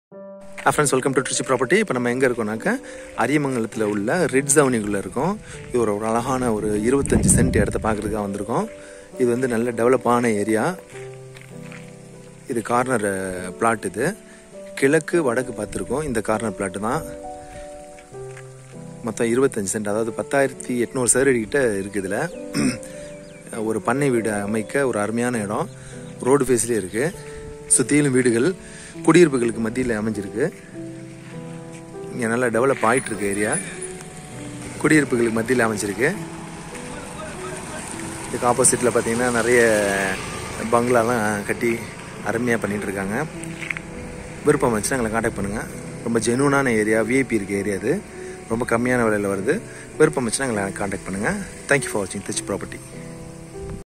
திருச்சி விஐபி ஏரியாவில் பண்ணை வீடு அமைக்க குறைந்த விலையில் அழகிய நிலம்
இடம் RIDZ AVENUE.

அளவு பத்தாயிரம் சதுர அடி.

குடியிருப்புகளுக்கு மத்தியில் அமைந்துள்ளது.

இடம் கிழக்கு வடக்கு பார்த்து அமைந்துள்ளது.

விஐபி ஏரியாவில் உள்ளது.

விலை ஒரு சதுர அடி ஆயிரத்து 500 மட்டுமே பேச்சுவார்த்தைக்கு உட்பட்டது.

தங்கள் விற்க நினைக்கும் சொத்துக்களை இலவசமாக விளம்பரம் செய்யவும் அல்லது புதிய சொத்துக்கள் வாங்க விற்க
TRICHY PROPERTY